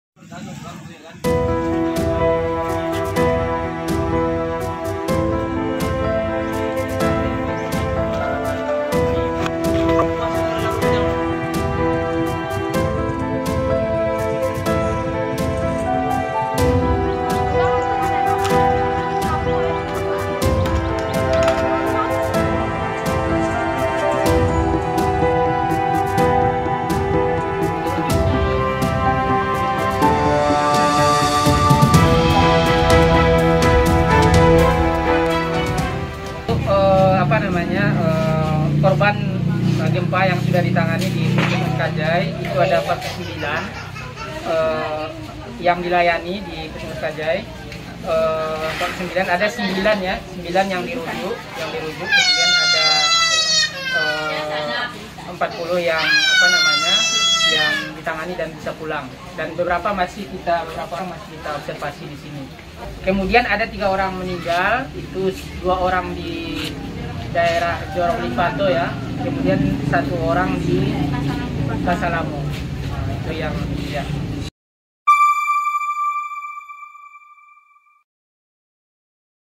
Korban Meninggal akibat Gempa Pasaman Barat Bertambah 3 Total Jadi 7 Orang
Data sementara dari BNPB melaporkan jumlah korban meinggal dunia akibat gempa bumi M 6,1 di Pasaman Barat bertambah 3 orang, sehingga total menjadi 7 orang, Jumat(25/2). Sementara, ribuan warga terdampak gempa mengungsi di 35 titik tenda pengungsian yang tersebar di Kecamatan Talamau, Kecamatan Pasaman dan Kinali, serta Kajai, Pasaman Barat.

Petugas gabungan dan relawan masih fokus pada pencarian serta evakuasi warga serta melakukan pelayanan kepada warga terdampak.

Kontributor: Budi Sunandar